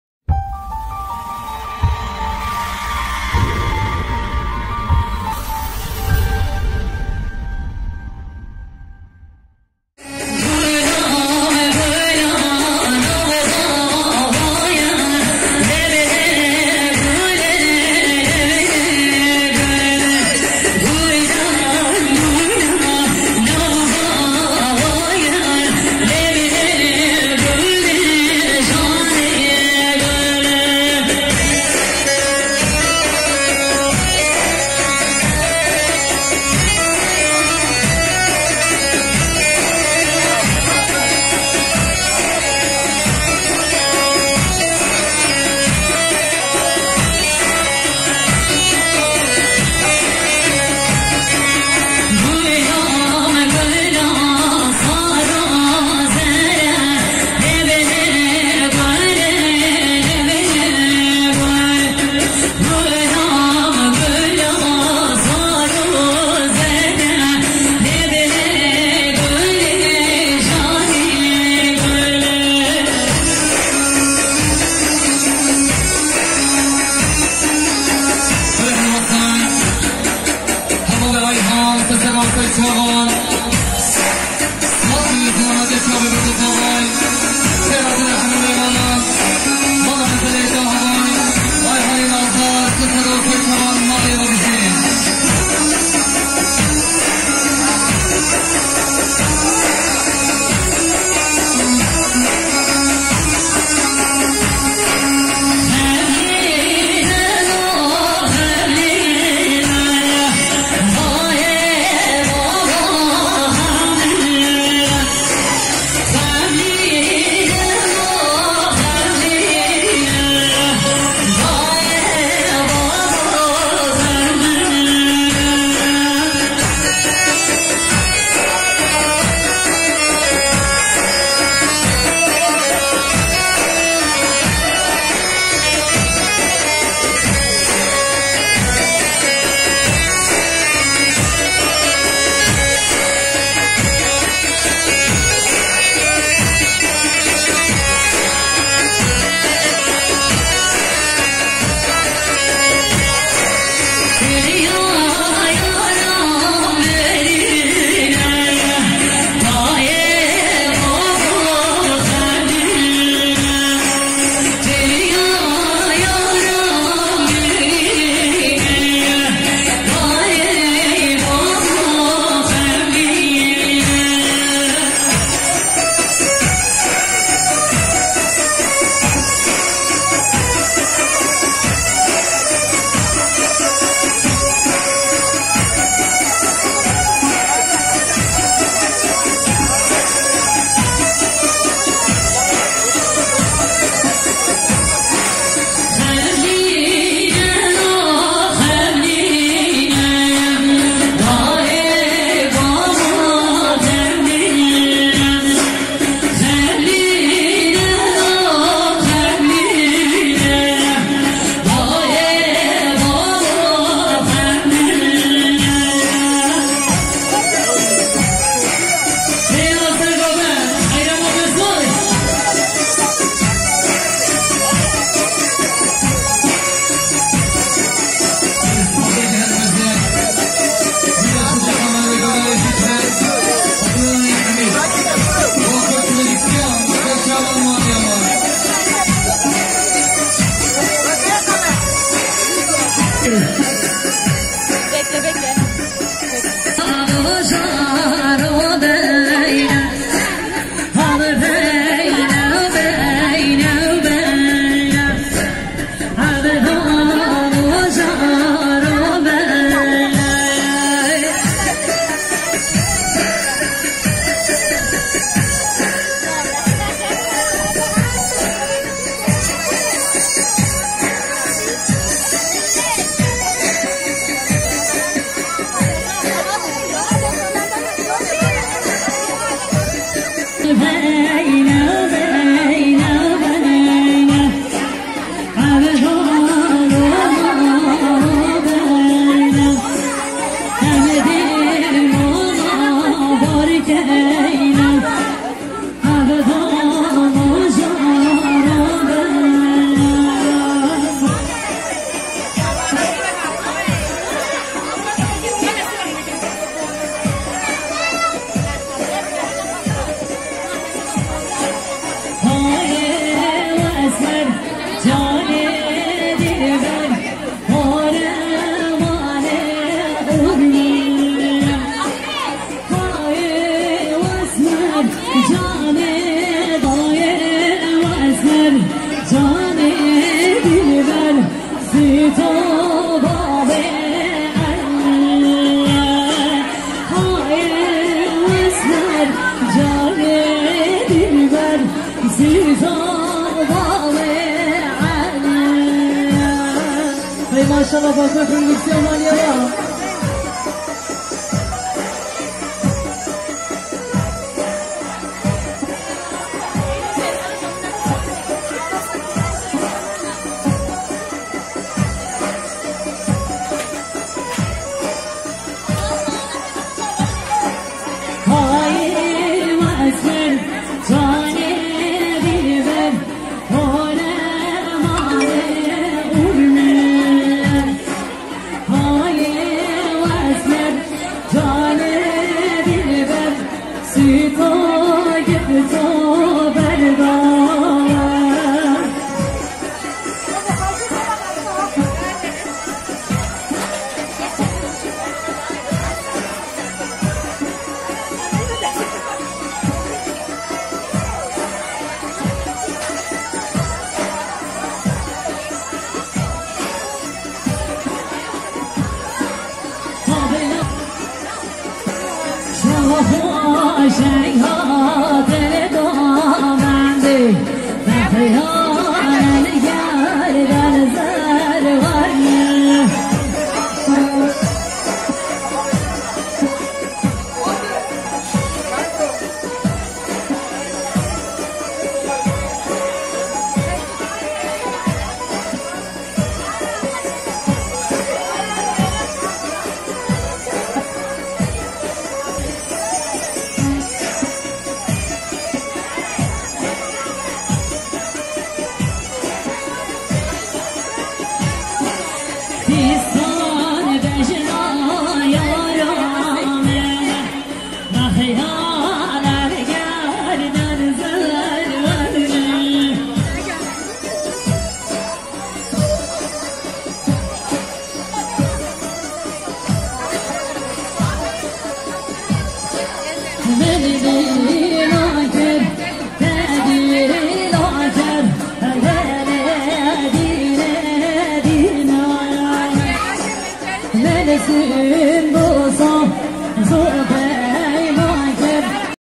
Hakkari düğünleri] taşbaşı köyü düğünleri [daru depine yeni halay 2021 kurdish wedding dance
🎬MED YAPIM PRODUCTİON🎬

↪beğeni yapmayı  unutmayınız↩
↪yorum yapmayı unutmayınız↩
🔊Arkadaşlar videomuzu kanalınıza kopyalıp paylaşmayınız lütfen YouTube tarafından Telif  hakkıyla o video kaldıralacaktır⚠